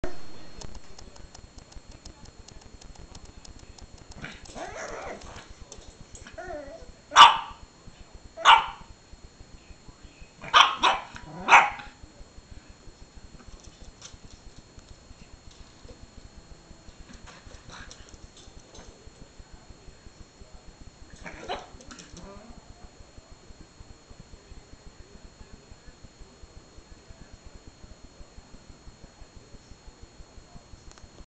Leaping Violet
Violet the Pug is growing and will soon be able to leap her play pen walls.